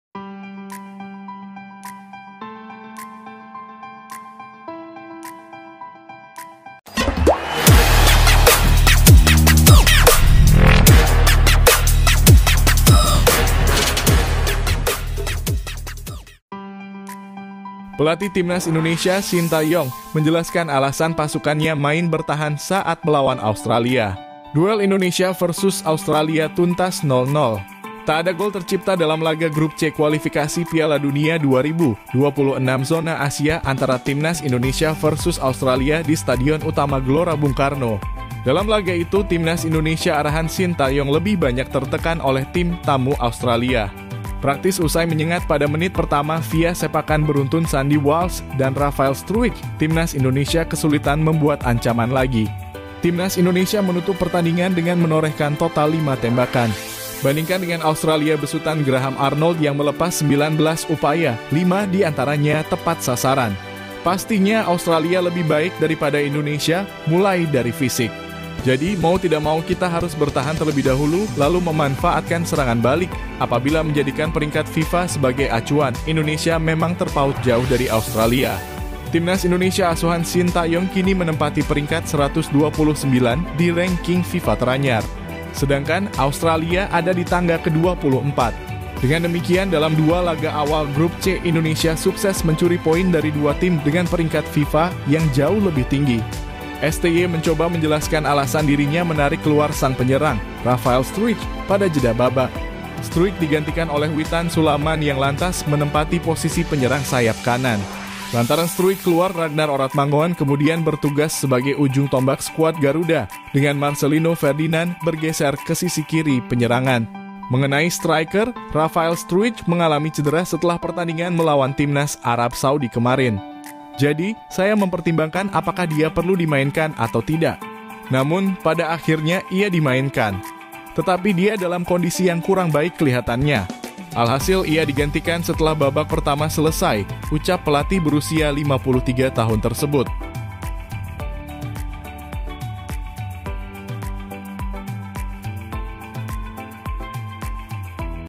Alasan dibalik Kokohnya pertahanan Timnas Indonesia,Cara latihan Defense Timnas saat Vs Australia
Ini Alasan dibalik Kokohnya pertahanan Timnas Indonesia,Cara latihan Defense Timnas saat Vs Australia

Tonton video hingga selesai supaya tau isi konten superliga

Tulis di kolom komentar jika ada saran